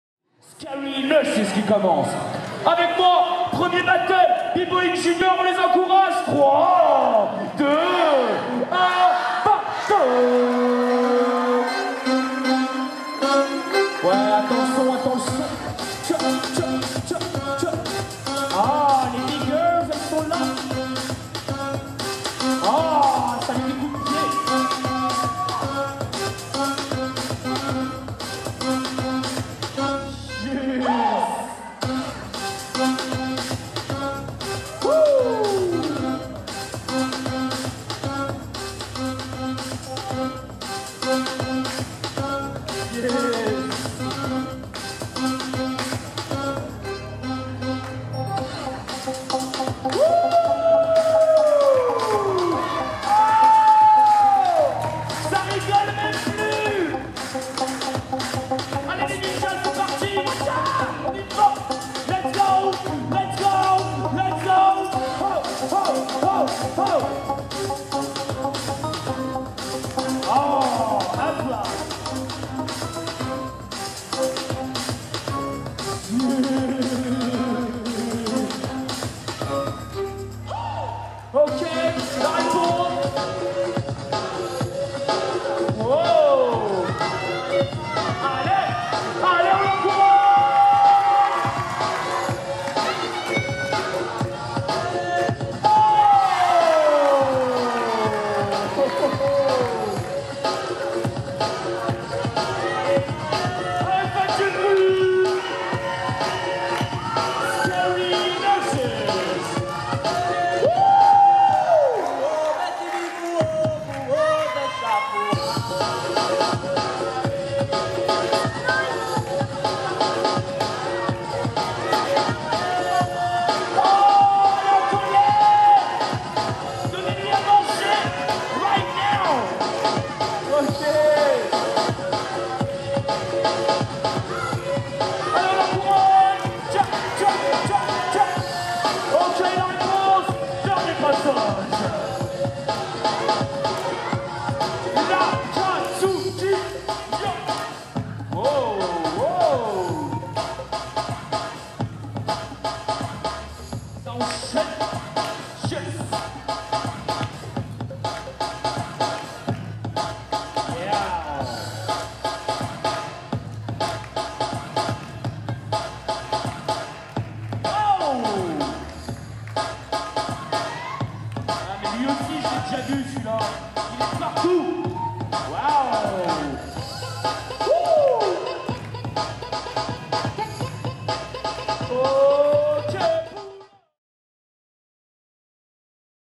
DEMI FINALE BBOYING JUNIOR (Scary nurses vs L'akatsuki)
Vidéo de la DEMI FINALE B.BOYING JUNIOR avec SCARY NURSES versus L'AKATSUKI !
Pour plus d'informations sur les cours proposés et les tarifs, rendez-vous sur notre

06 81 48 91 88

Peace !